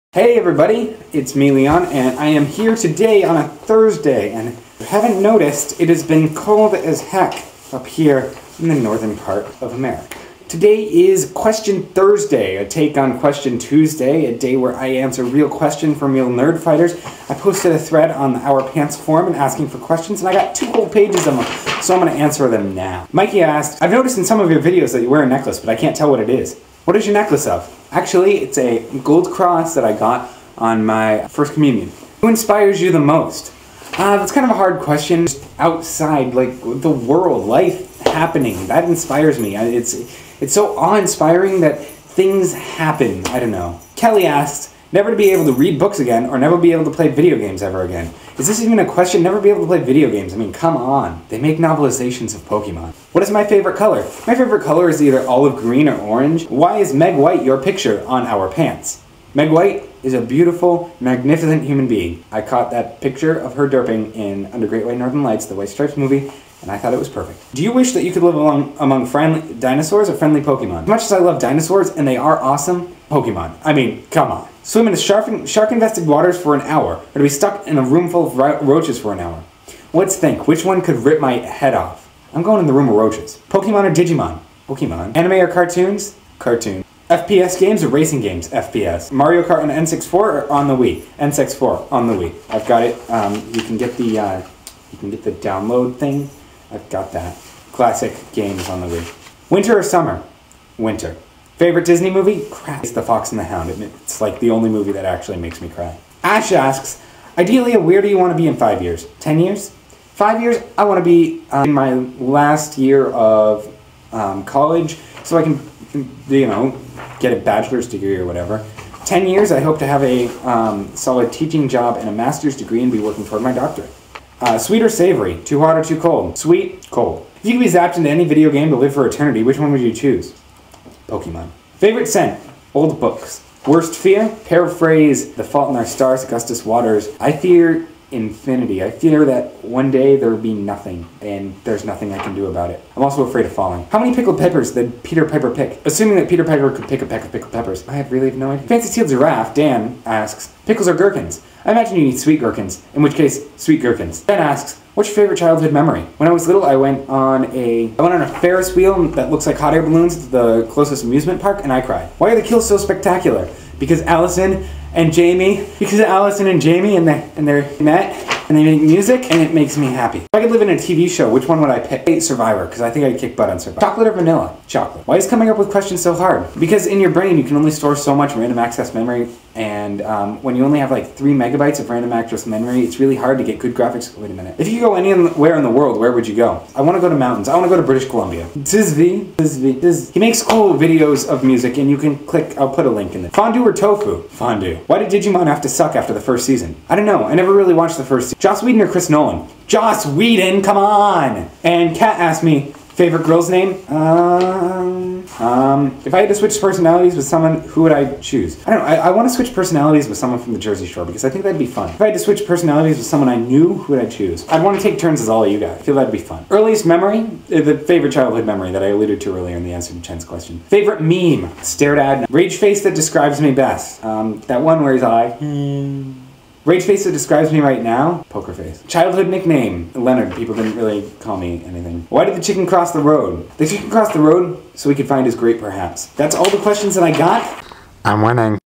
January 24th: Question Thursday!
In which LeoN answers real questions from real nerdfighters.